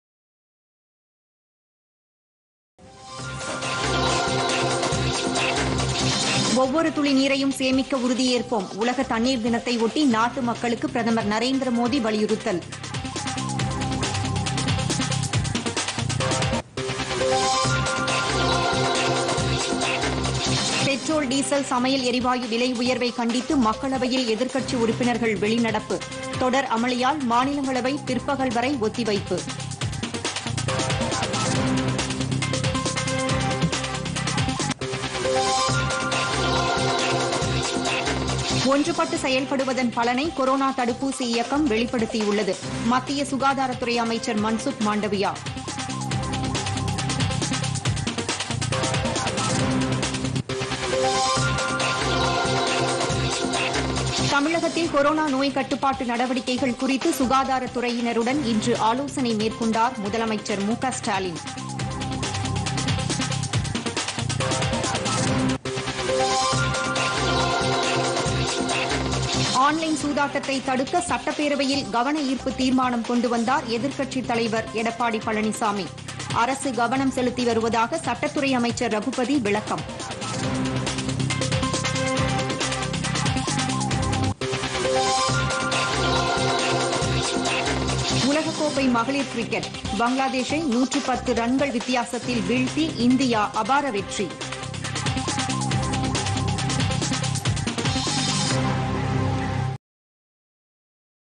பொதிகை பிற்பகல் 2.00 மணி தலைப்புச் செய்திகள் [22.03.2022]
1) ஒவ்வொரு துளி நீரையும் சேமிக்க உறுதியேற்போம் - உலக தண்ணீர் தினத்தையொட்டி நாட்டு மக்களுக்கு பிரதமர் நரேந்திர மோதி வலியுறுத்தல்.

2)  பெட்ரோல், டீசல், சமையல் எரிவாயு  விலை உயர்வைக் கண்டித்து மக்களவையில் எதிர்க்கட்சி உறுப்பினர்கள் வெளிநடப்பு - தொடர் அமளியால் மாநிலங்களவை பிற்பகல் வரை ஒத்திவைப்பு.

3)  ஒன்றுபட்டு செயல்படுவதன் பலனை கொரோனா தடுப்பூசி இயக்கம் வெளிப்படுத்தியுள்ளது - மத்திய சுகாதாரத்துறை அமைச்சர் மன்சுக் மாண்டவியா.

4)  தமிழகத்தில்  கொரோனா நோய்க்கட்டுப்பாடு நடவடிக்கைகள் குறித்து சுகாதாரத் துறையினருடன் இன்று ஆலோசனை மேற்கொண்டார்  முதலமைச்சர் மு க ஸ்டாலின்.

5)  ஆன்லைன் சூதாட்டத்தை தடுக்க சட்டப்பேரவையில் கவன ஈர்ப்புத் தீர்மானம் கொண்டு வந்தார் எதிர்க்கட்சித் தலைவர் எடப்பாடி பழனிசாமி - அரசு கவனம் செலுத்தி வருவதாக  சட்டத்துறை அமைச்சர் ரகுபதி விளக்கம்.

6)  உலகக் கோப்பை மகளிர் கிரிக்கெட் - பங்களாதேஷை 110 ரன் வித்தியாசத்தில் வீழ்த்தி இந்தியா
அபார வெற்றி.